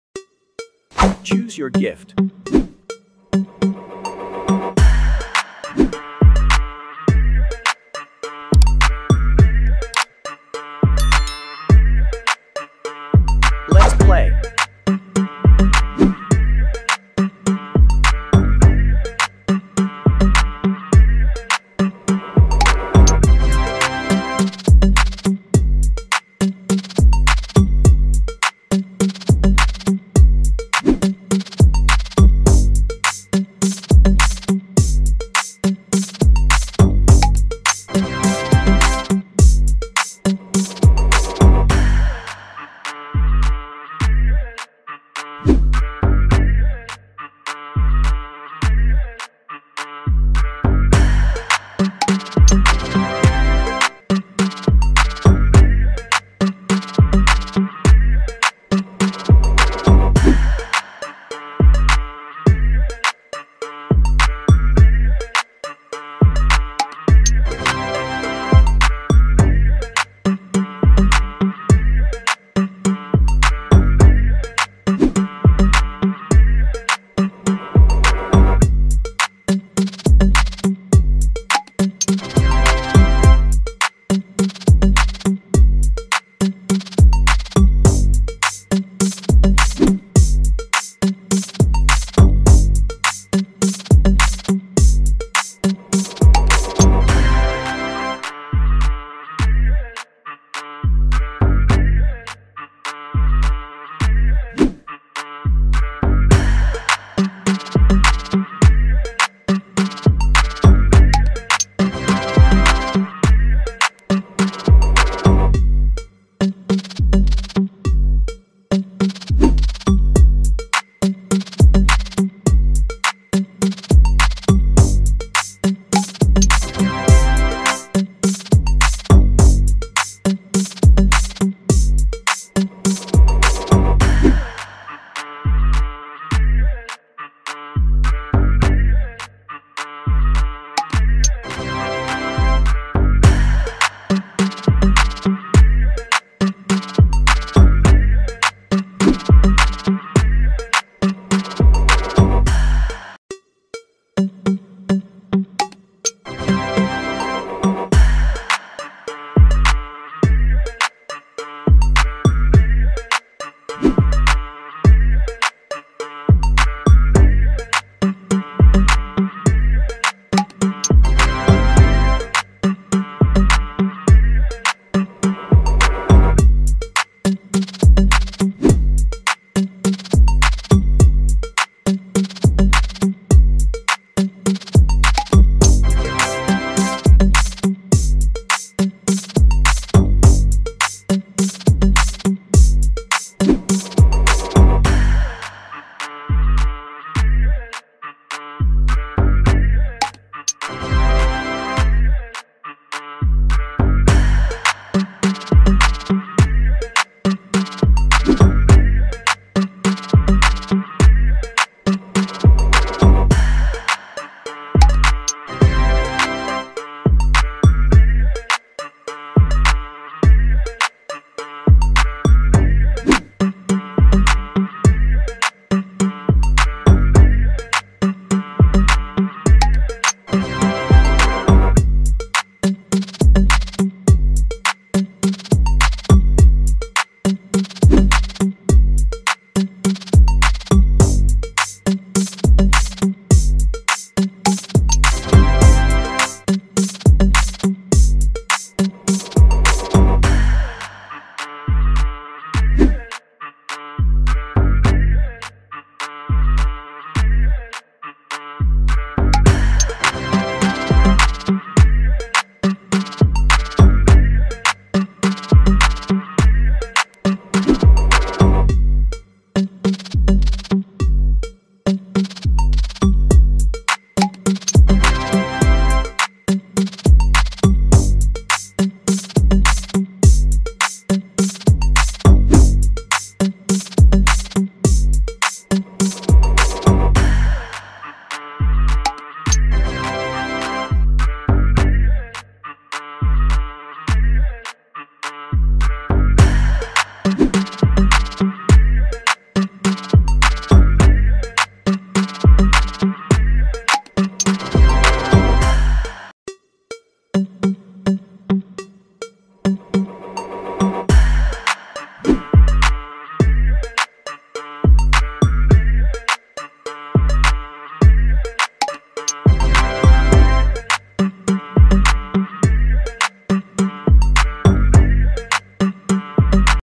Black vs White 🖤🤍 Choose Your Gift 🎁 Fun Gifts 🌟 by Chasing Career
Black vs White 🖤🤍  Choose Your Gift 🎁  Fun Gifts 🌟
Choose your gift and see what surprise is in the box!
I hope you have fun with your friends and family and that you can have a good laugh together.